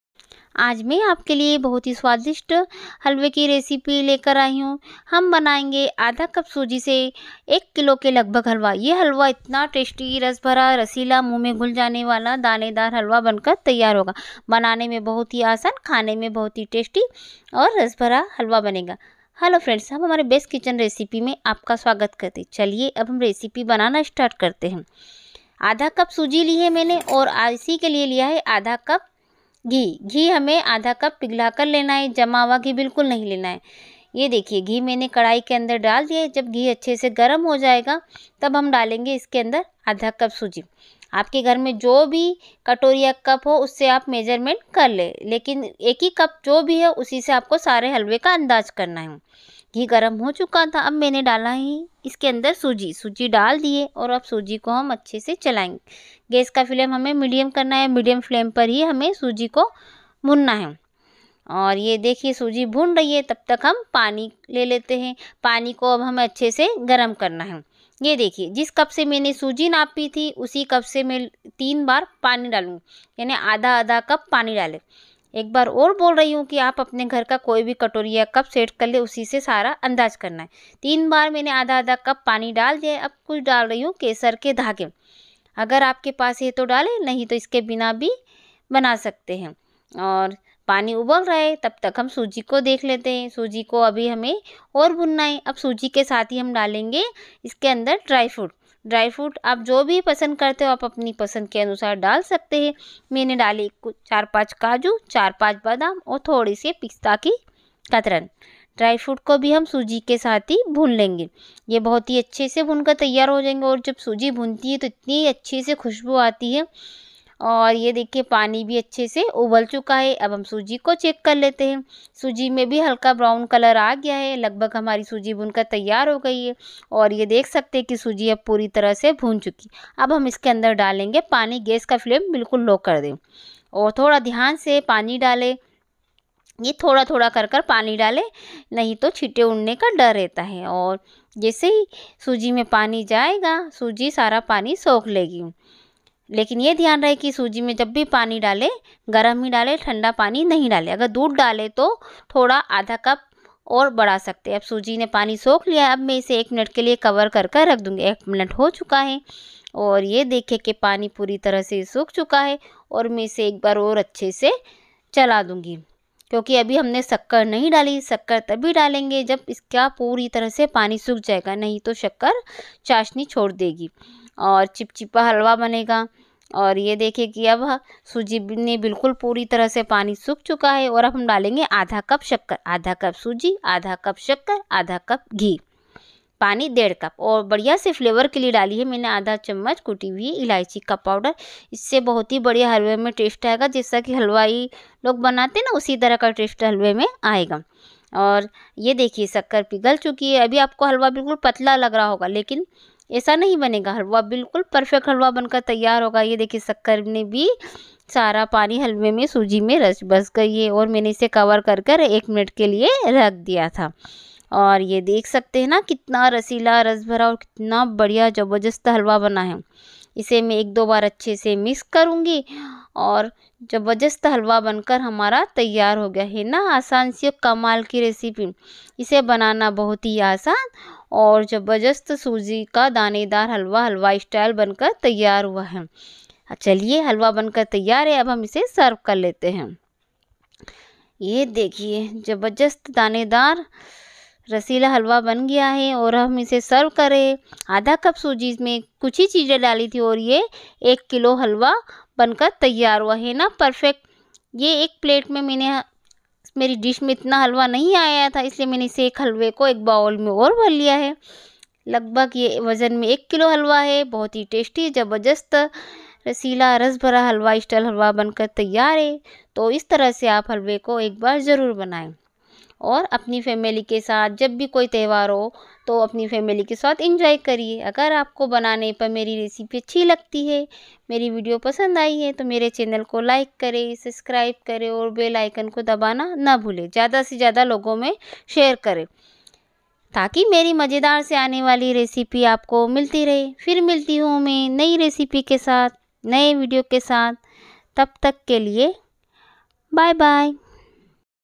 बिना चाशनी,सही तरीके से 1/2 cup सूजी और कुछ चीजों से 1 kg दानेदार हलवा बनाए।Sooji Halwa। Rawa Kesari
Sooji ( fine )  -1/2  Cup
Sugar -  1/2  Cup
Desi Ghee -  1/2  Cup
Water  -      1  Or 1/2 Cup
Dry fruit
Cardmon Powder  -  1/2 tsp
Kesar -  10 -12 dhaga